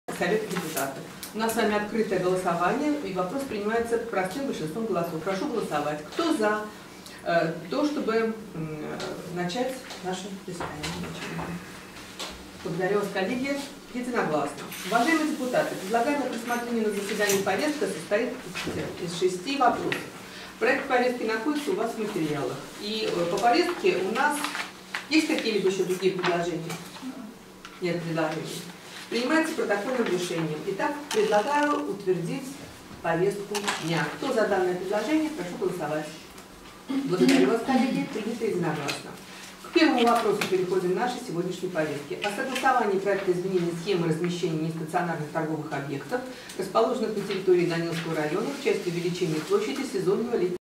В Даниловском районе состоялось внеочередное заседание Совета депутатов
Внеочередное заседание Совета депутатов состоялось в муниципальном округе Даниловский в минувший вторник, 6 августа.